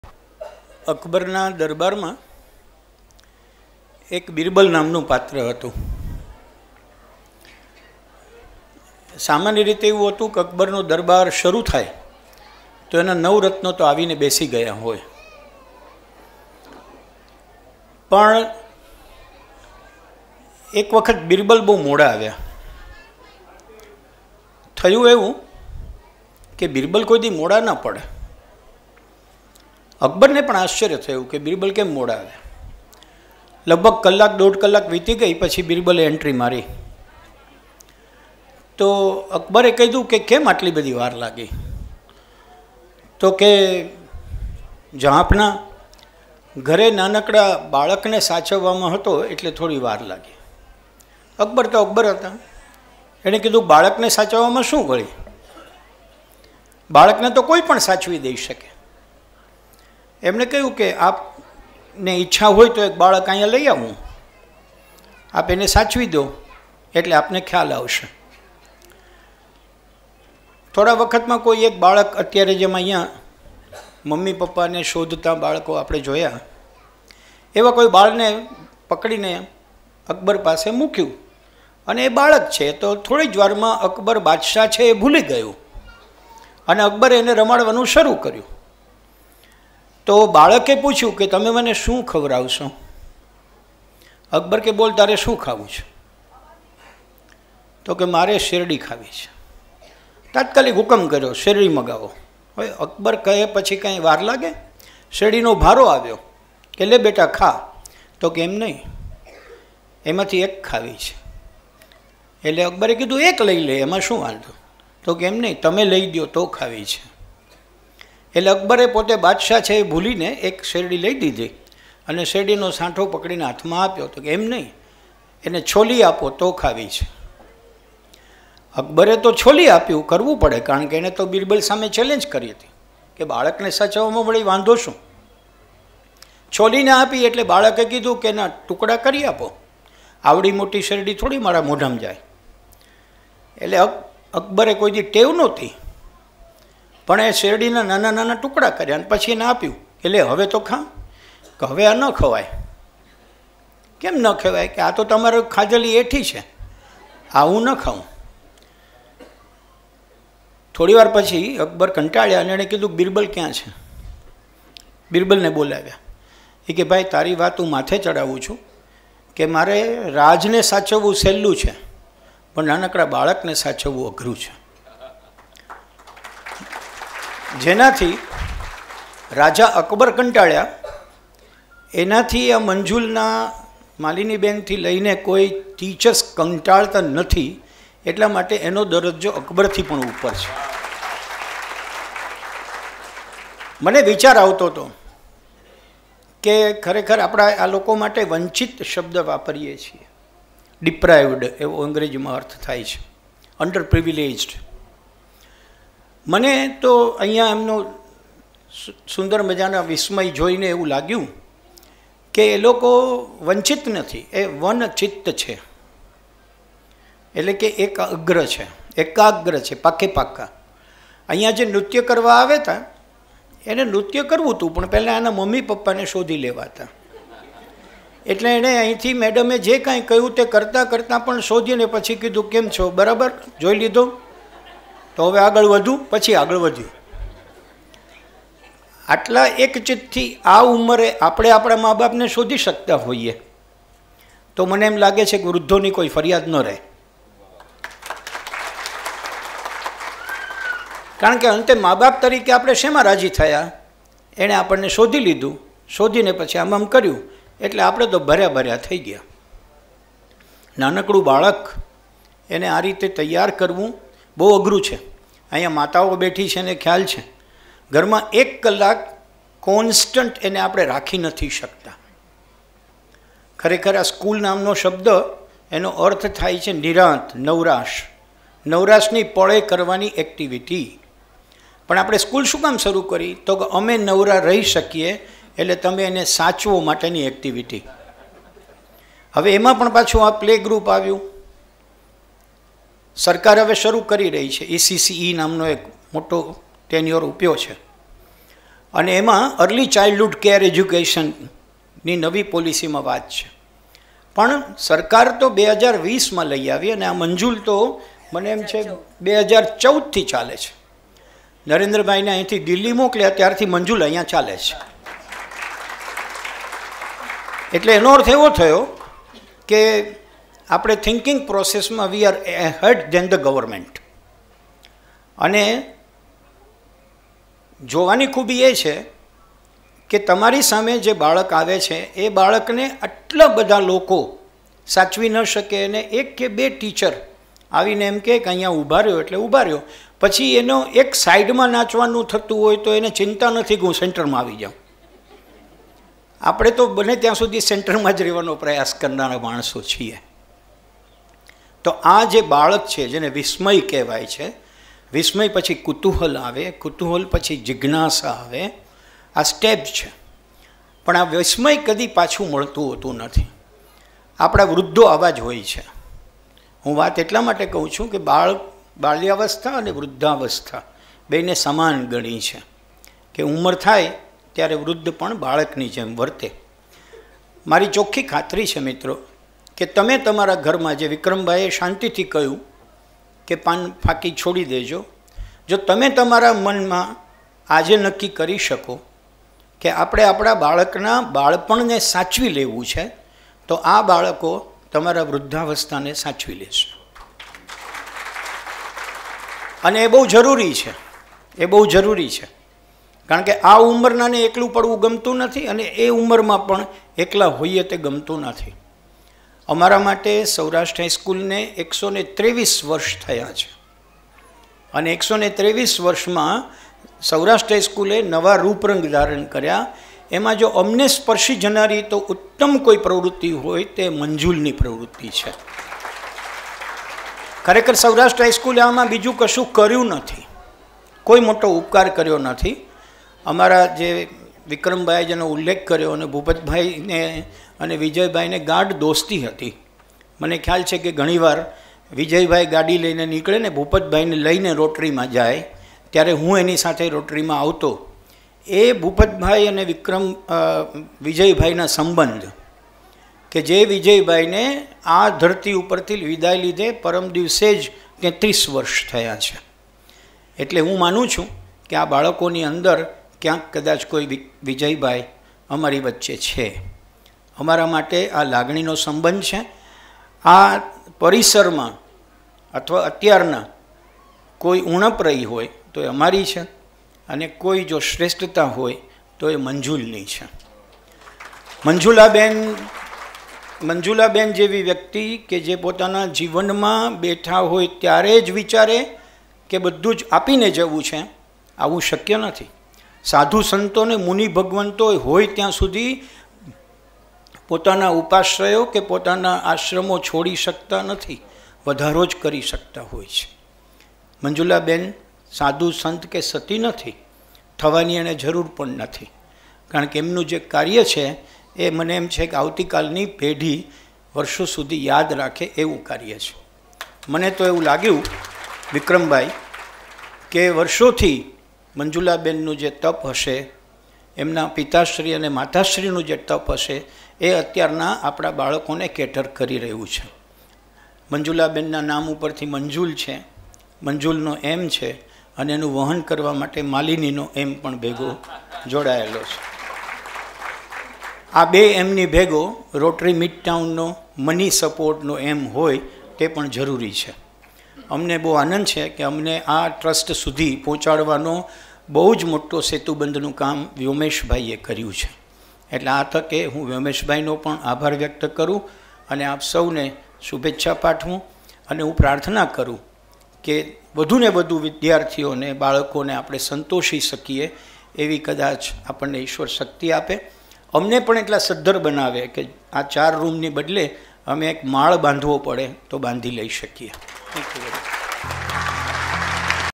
Manjul : વંચિતોને આધુનિક શિક્ષણ આપતી PreSchool::
સૌરાષ્ટ્ર હાઈસ્કૂલ ટ્રસ્ટ ની નિસ્બતભરી સેવા
Annual Function માં ભદ્રાયુ વછરાજાની નું સટીક વક્તવ્ય
Bhadrayu Vachhrajani